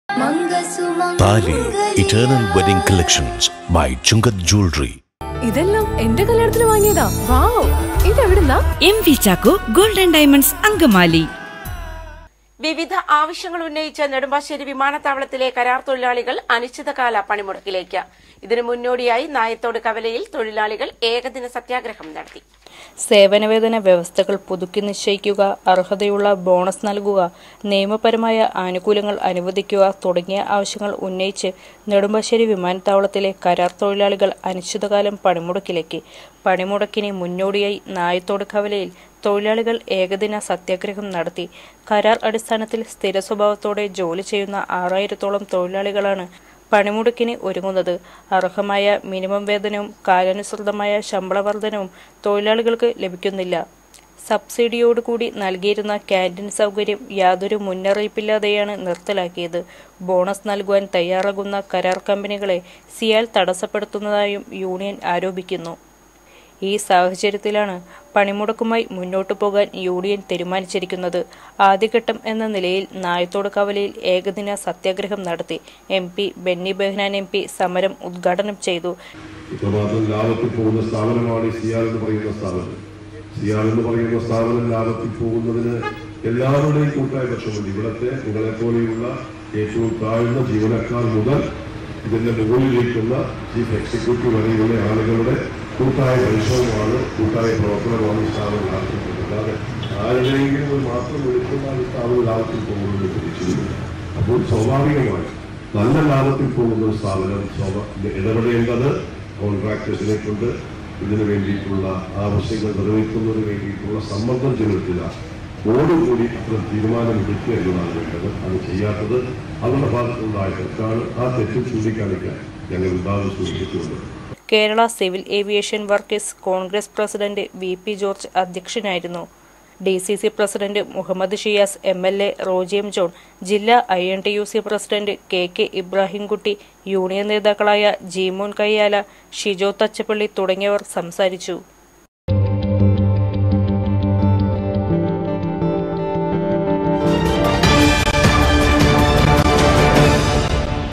നെടുമ്പാശ്ശേരി വിമാനത്താവളത്തിലെ കരാർ അടിസ്ഥാന തൊഴിലാളികൾ അനിശ്ചിതകാല പണിമുടക്കിലേക്ക്
ACTVNewsAngamaly # angamaly

2023 AUGUST 18 th NEWS